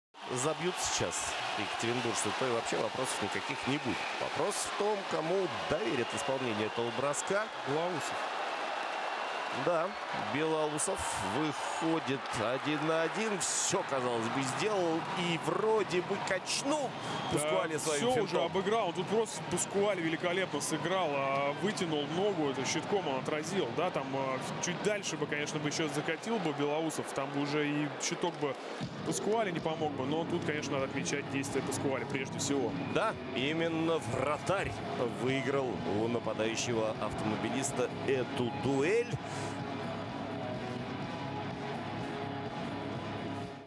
Паскуале спасает лезвием конька / Pasquale saves it with his skate
Паскуале не дал Белоусову реализовать штрафной бросок

Pasquale saves it with his skate